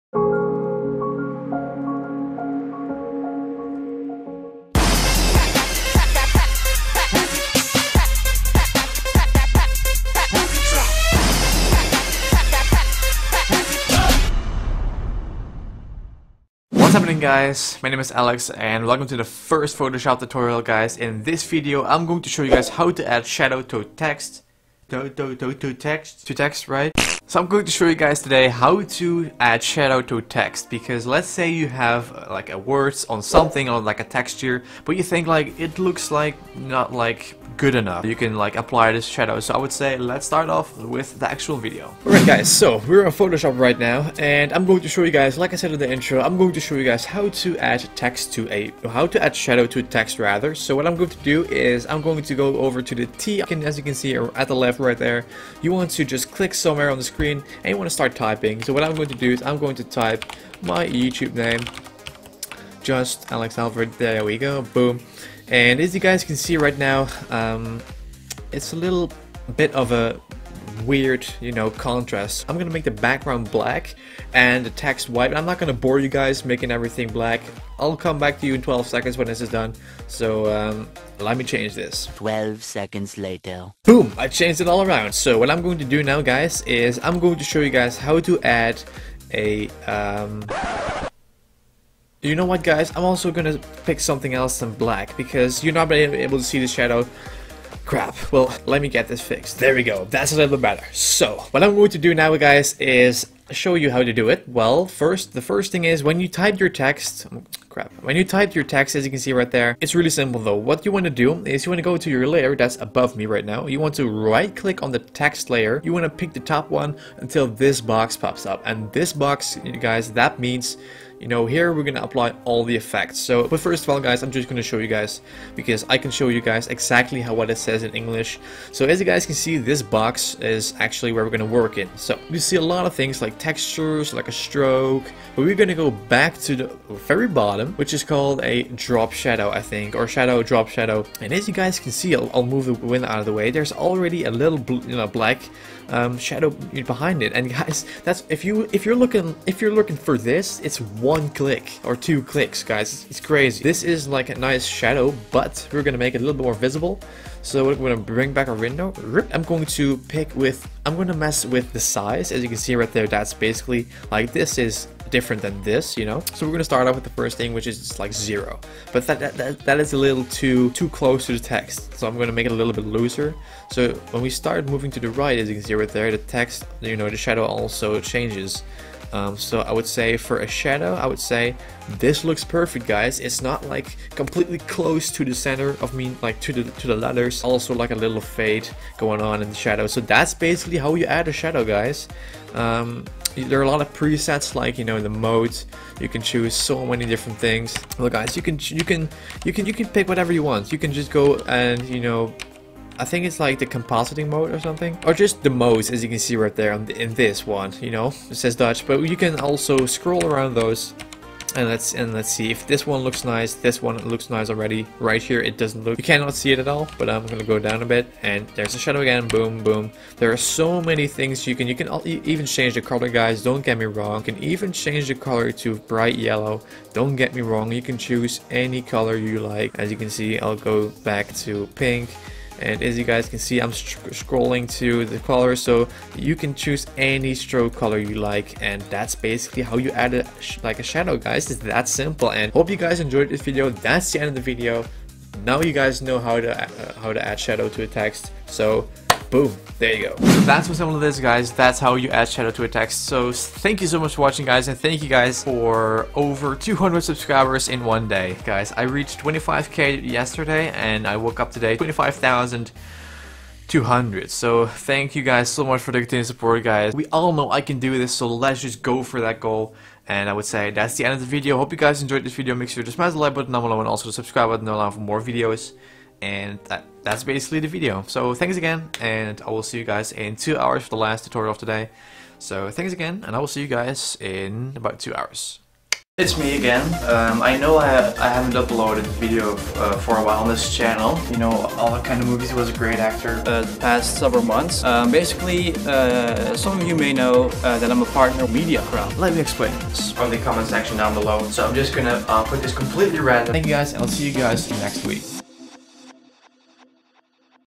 Adobe Photoshop CC: How To Add Shadow To Text EASILY - Tutorial #33
- - In this video I'll be showing you how to add shadow to text.
It's done in the photo editing software called Photoshop CC 2014. You can use Adobe Photoshop CC, CS4, CS5, and CS6.

~ JustAlexHalford